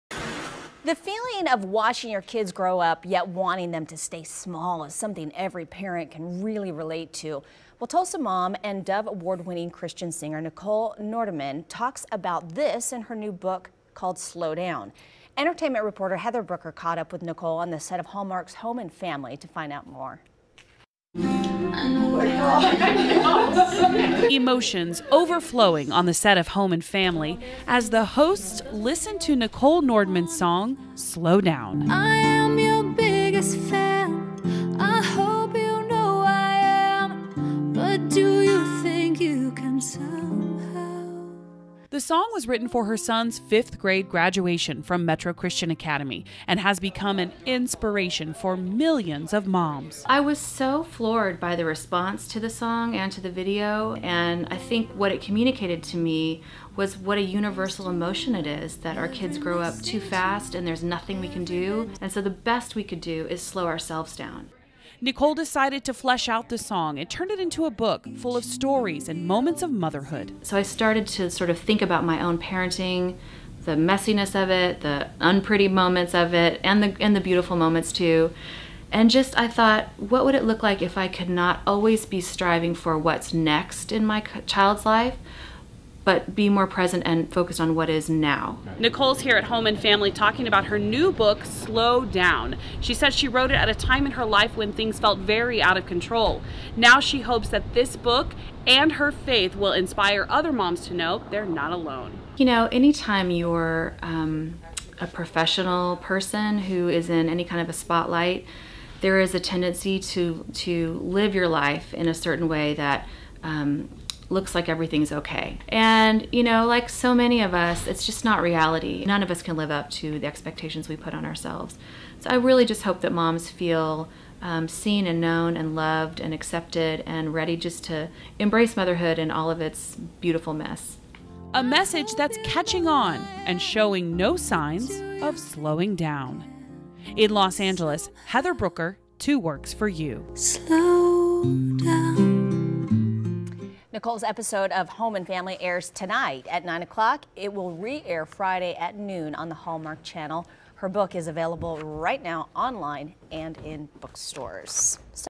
Singer/Songwriter Nichole Nordeman Talks About Her New Book 'Slow Down'
KJRH 2 Works For Your reporter Heather Brooker caught up with Singer/Songwriter Nichole Nordeman on the set of Home and Family to talk about her new book 'Slow Down'. You can read more about it and watch her video at: